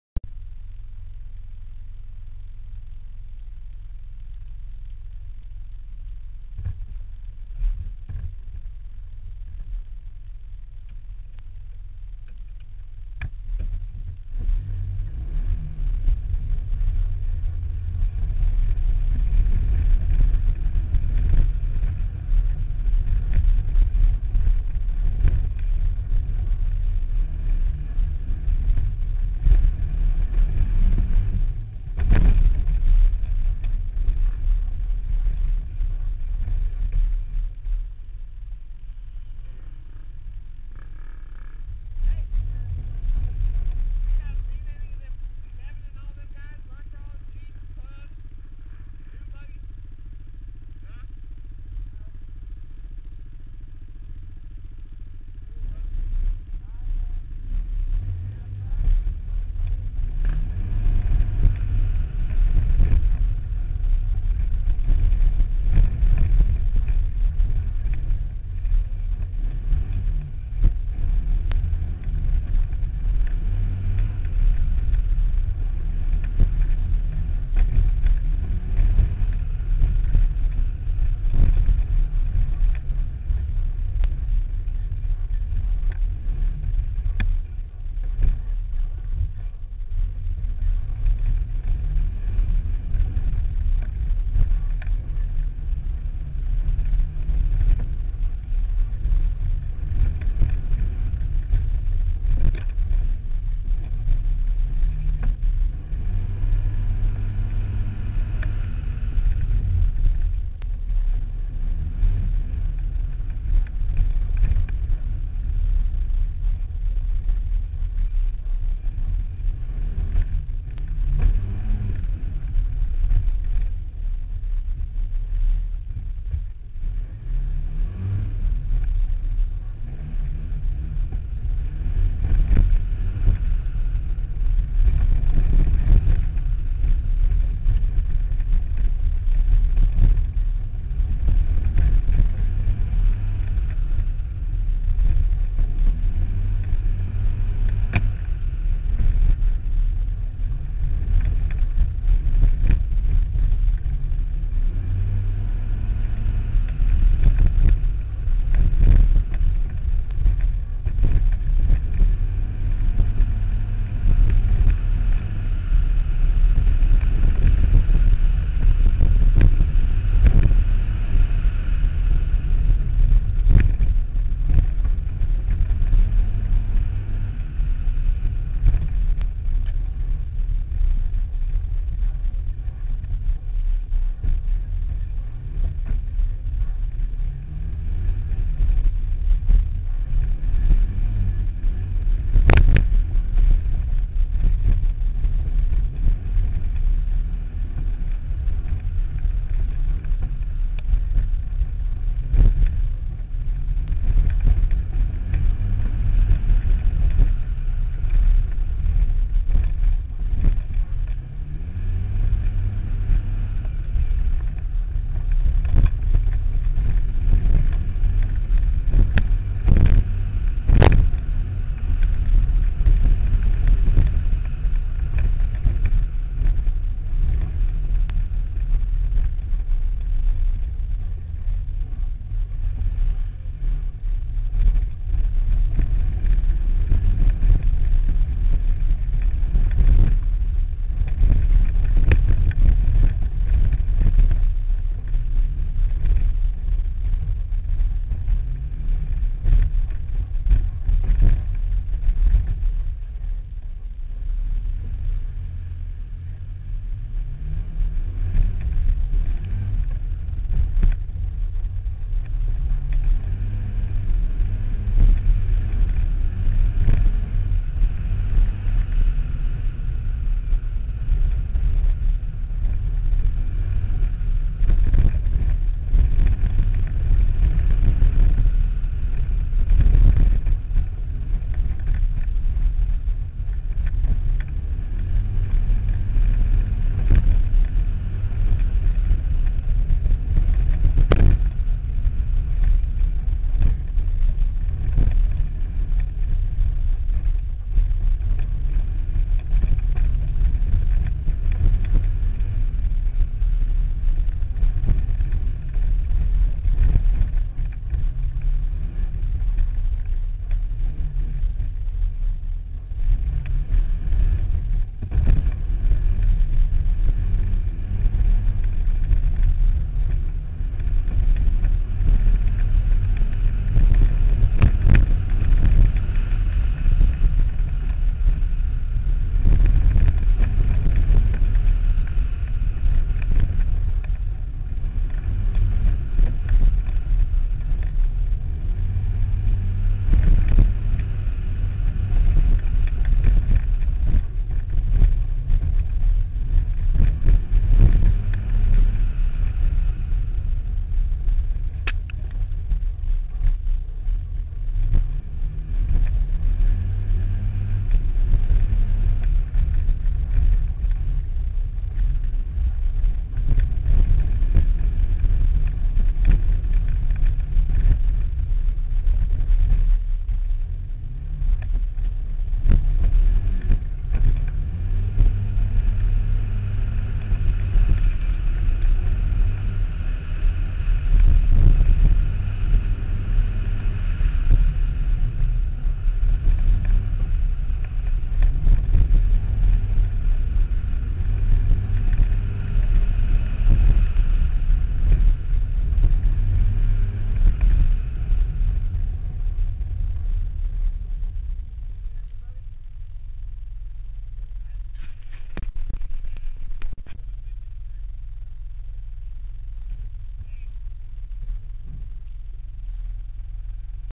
Randolph ORV "A RZR Perspective"
Randolph Kansas ORV park in a dat gum 2010 RZR-S